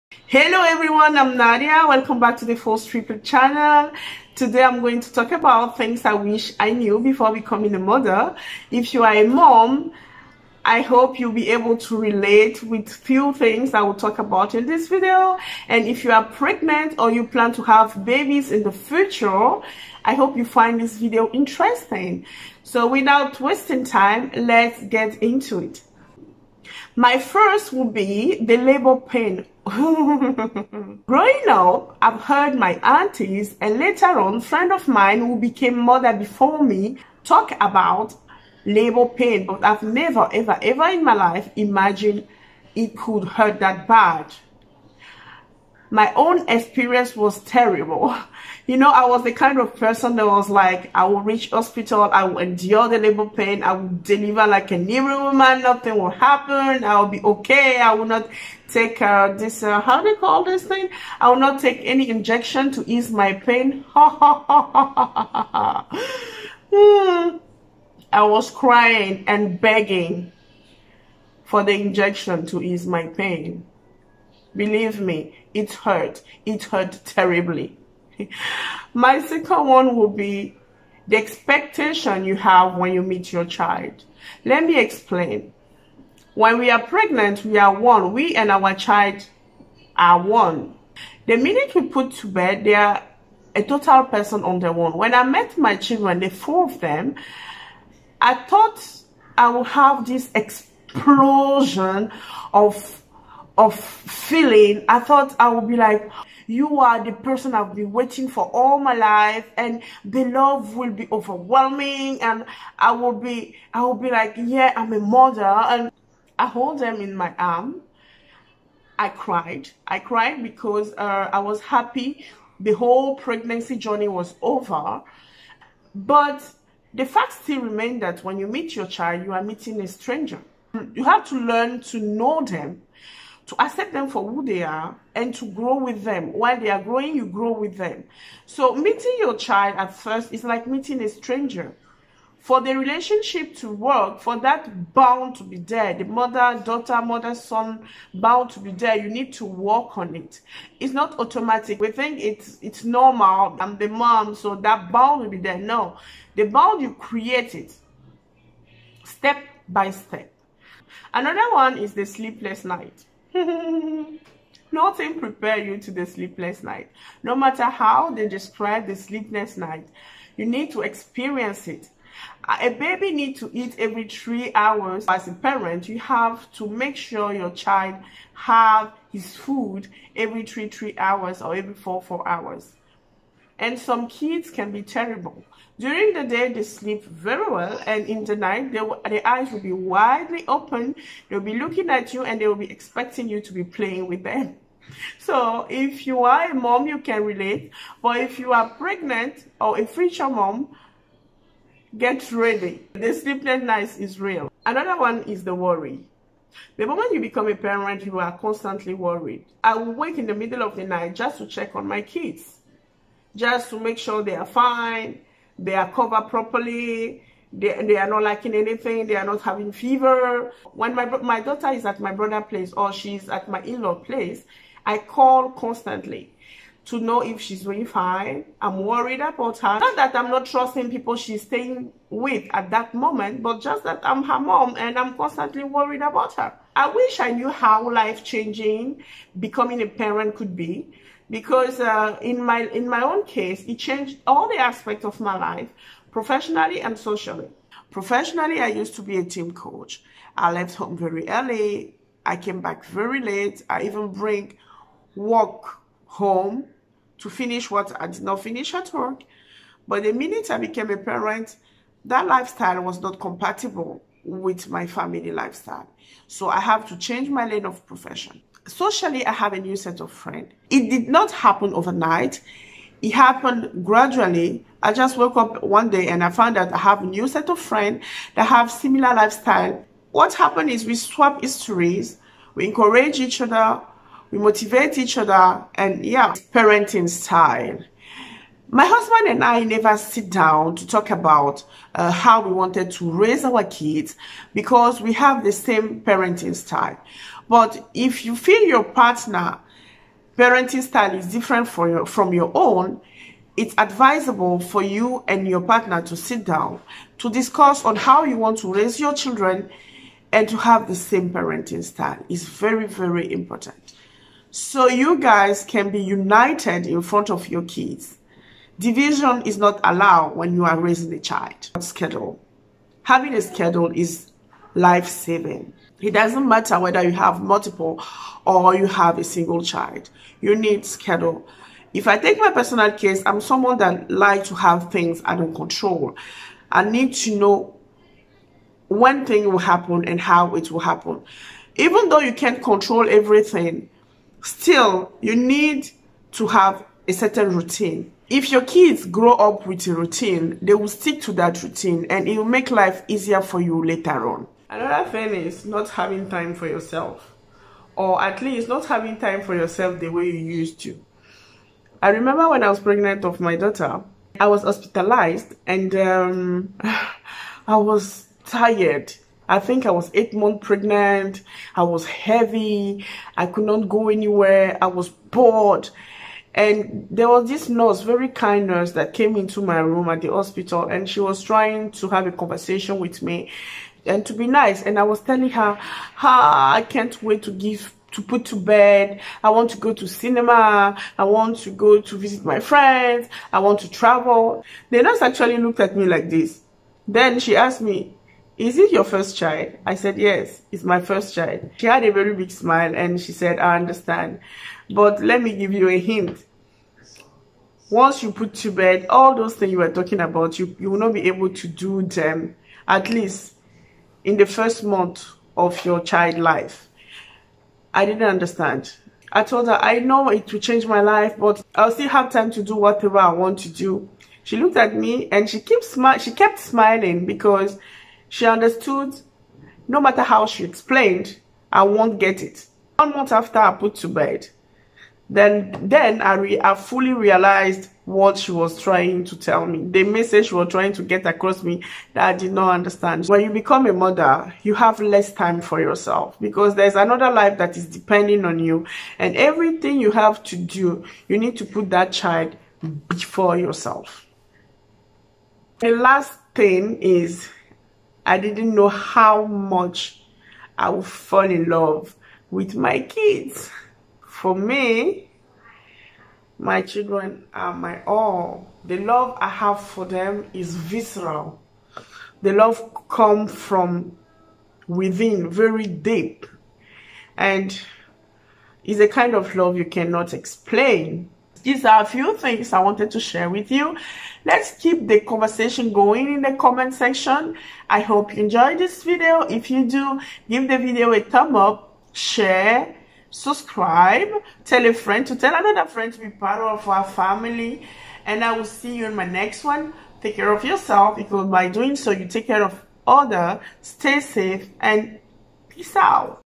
First time mom lessons. Expectations vs realities
In this video i am talking about the life changing lessons I learned as being a first time mom. You are never fully prepared to become a parent. No matter how ready you are, when the motherhood journey start you will feel like someone is sweeping you off your feet. Here are few of the things they don't tell you about parenthood and how it changes your life around.